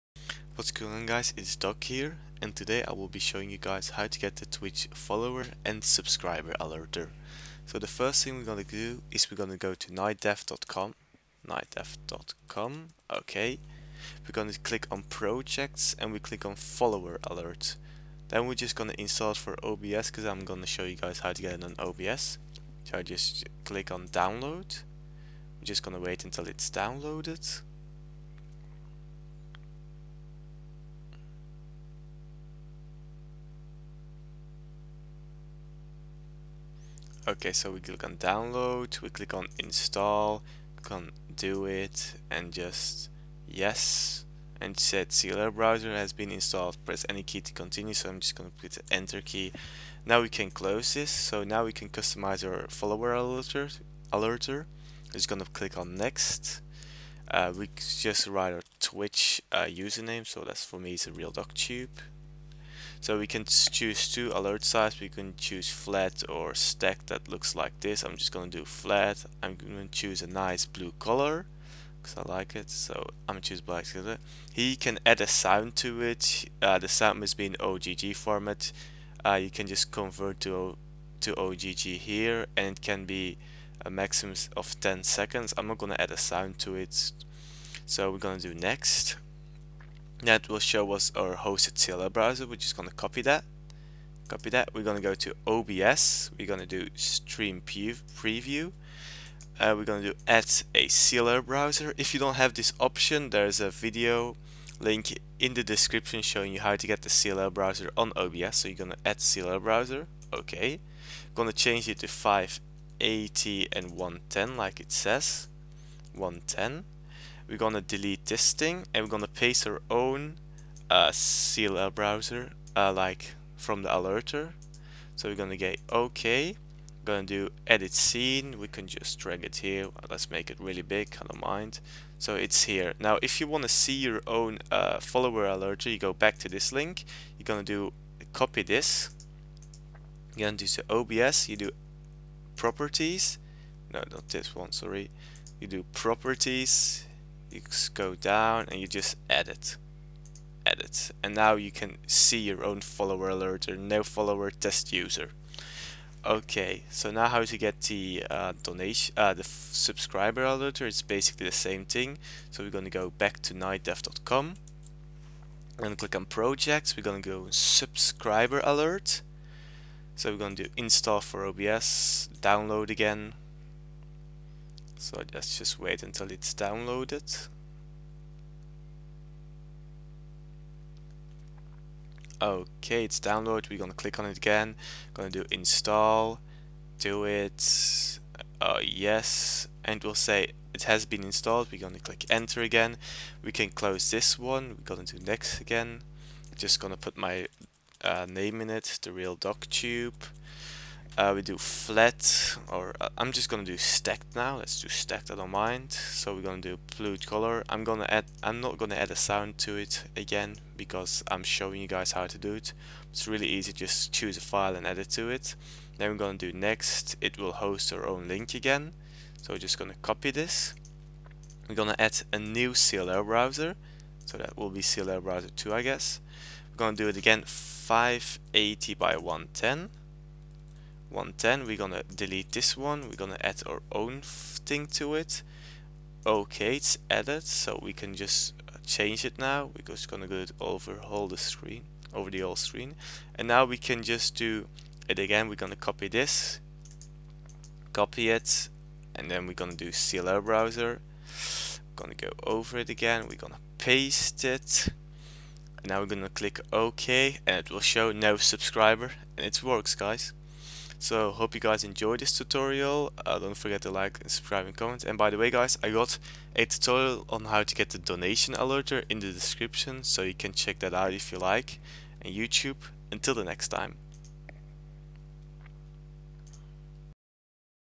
Twitch Follower & Subscriber Alert Tutorial
A tutorial showing how to get the twitch follower and subscriber alerter for obs using

Twitch Follower Alert
Twitch Follower Alert